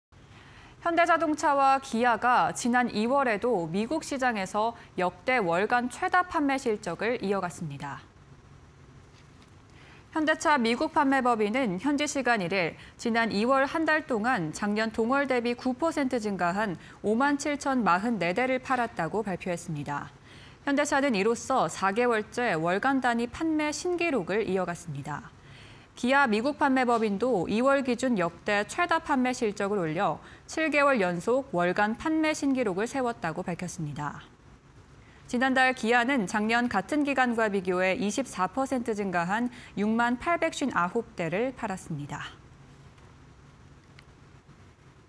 현대차·기아, 미국 자동차 시장서 역대 2월 최다 판매 기록 (20230302)
현대자동차와 기아가 지난 2월에도 미국 시장에서 역대 월간 최다 판매 실적을 이어갔습니다.
현대차 미국판매법인(HMA)은 현지시간 1일 지난 2월 한 달 동안 작년 동월 대비 9% 증가한 5만7044대를 팔았다고 발표했습니다. 현대차는 이로써 4개월째 월간 단위 판매 신기록을 이어갔습니다.
기아 미국판매법인(KA)도 2월 기준 역대 최다 판매 실적을 올려 7개월 연속 월간 판매 신기록을 세웠다고 밝혔습니다.
지난달 기아는 작년 같은 기간과 비교해 24% 증가한 6만859대를 팔았습니다.